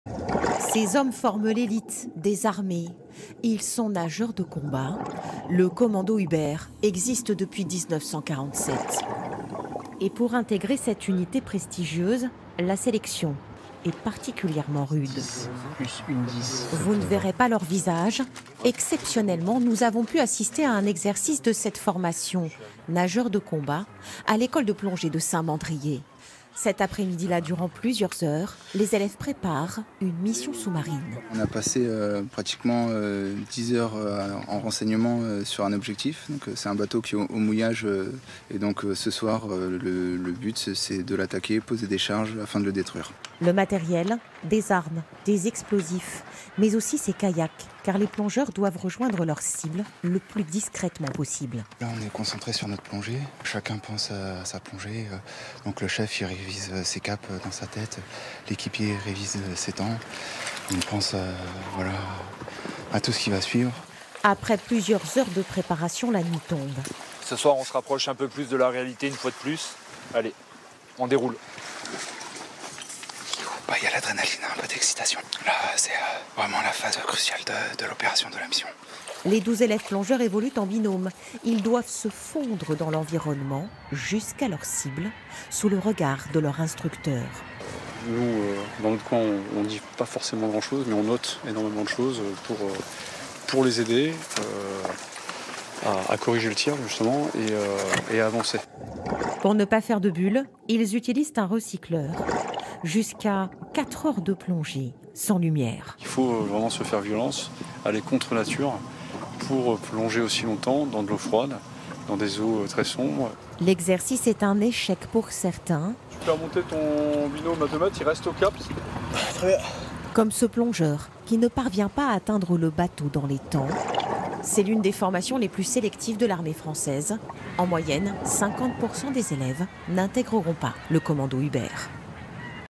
Aux côtés des plongeurs d'élite du mythique commando Hubert de la Marine
Les plongeurs du commando Hubert de la marine nationale suivent une formation très sélective. Ces nageurs peu connus travaillent dans l'ombre.
TF1 a assisté à une séance de formation spectaculaire en Méditerranée.